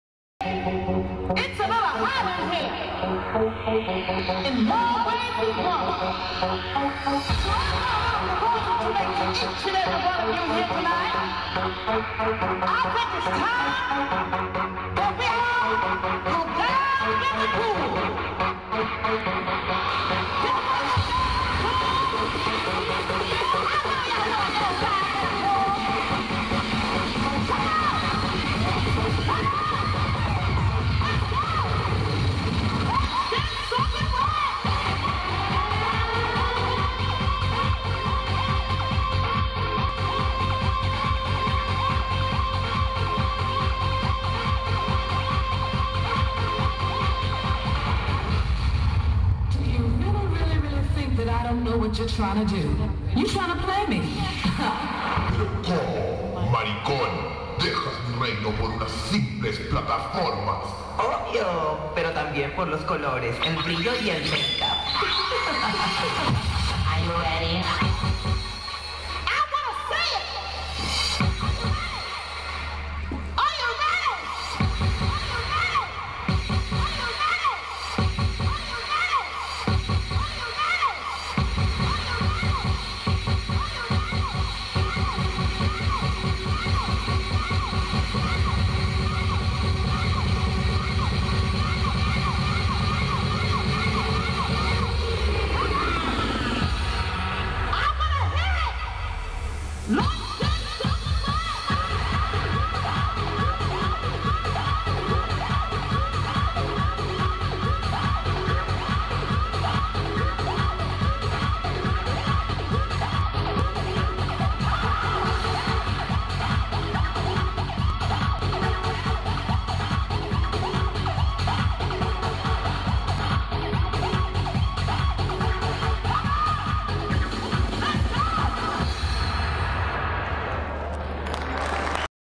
Urán Drag Queen
' Urán Drag Queen En La carpa de Checopete Presentando : " Los años Dorados de La tia Carlina 2.0 " ; Vallenar , Urán Drag Queen Uno de los Mejores Show de la Noche :)